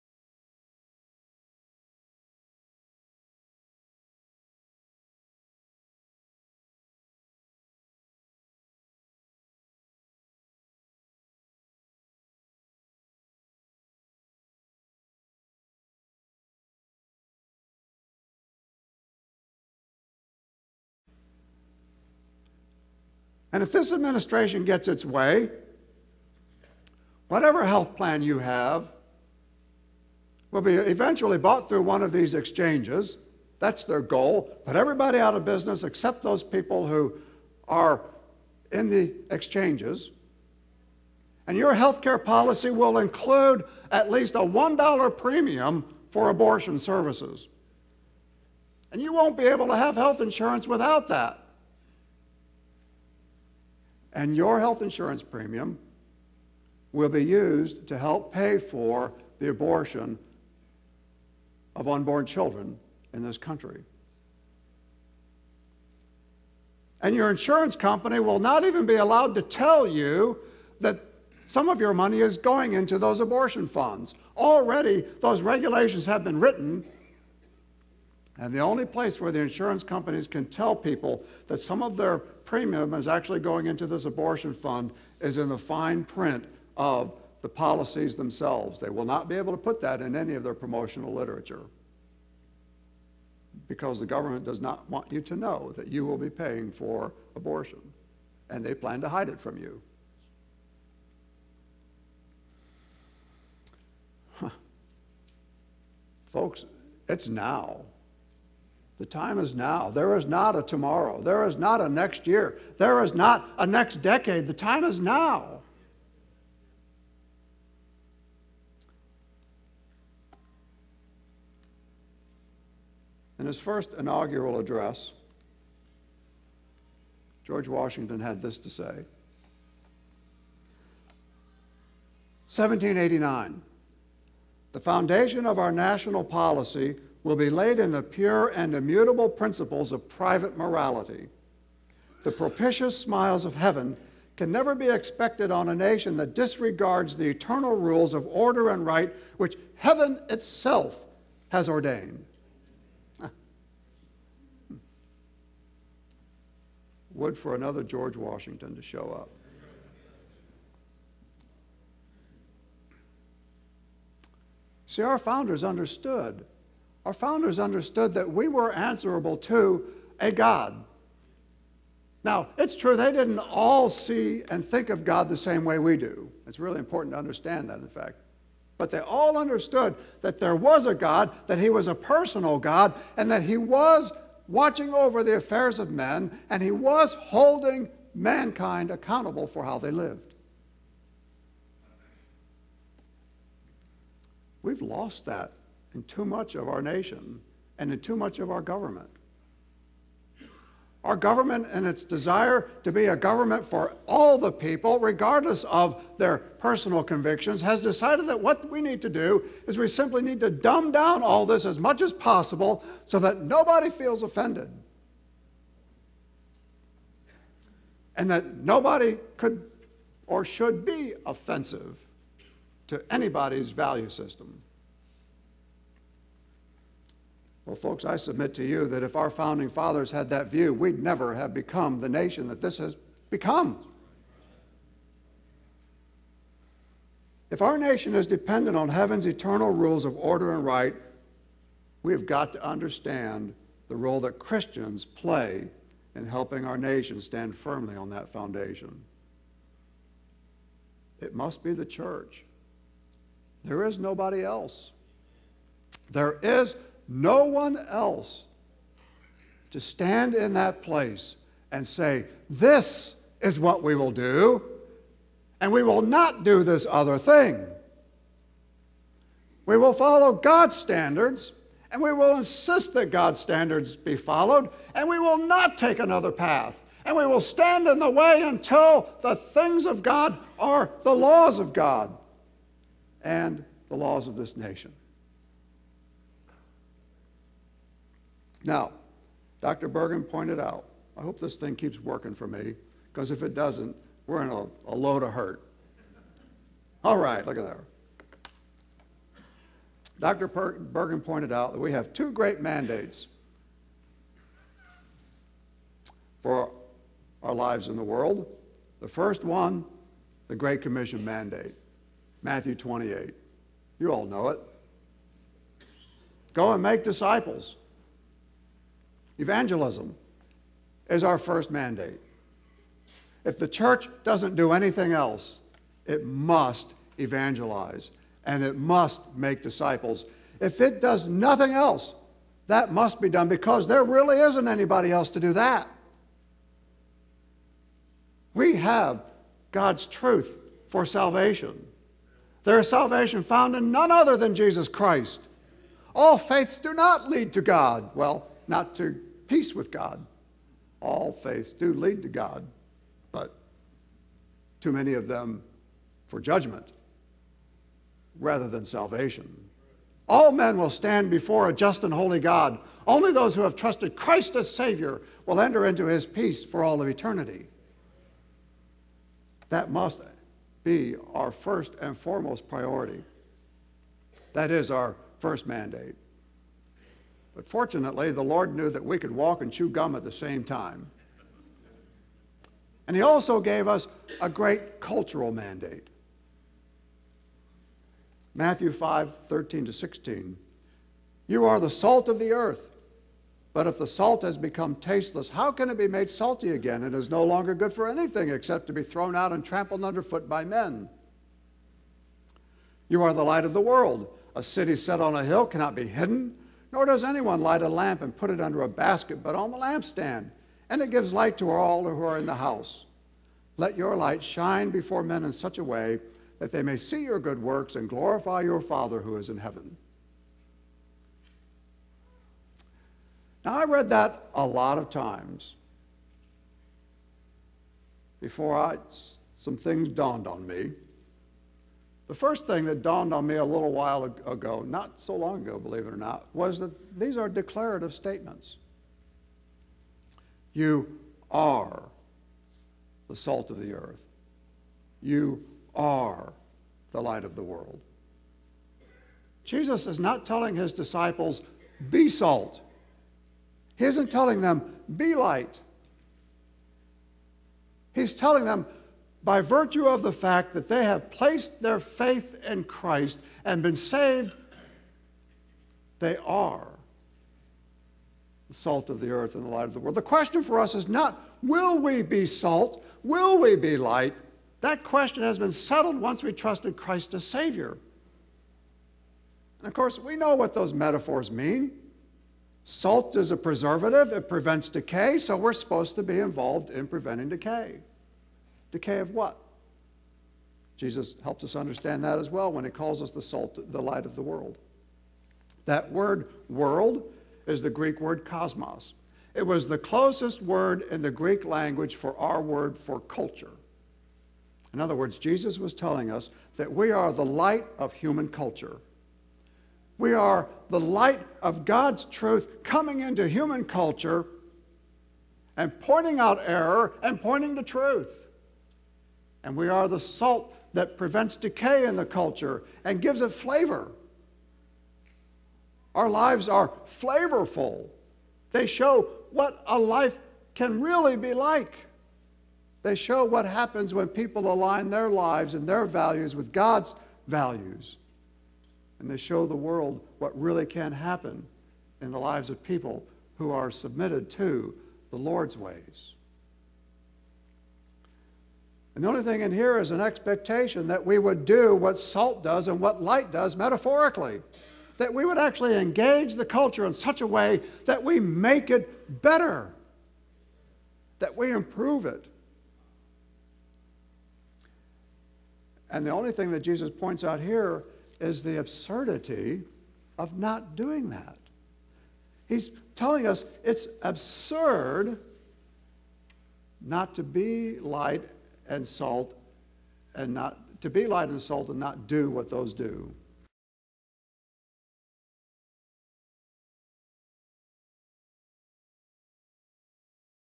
Dr. Barret Duke, Session 1-C
Dr. Barret Duke continues his discussion of religious liberty during the Worldview Conference at Memorial Baptist Church, Jefferson City, MO - March 29th, 2012. Video provided by The Christian Life Commission and The Pathway - entities of the Missouri Baptist Convention.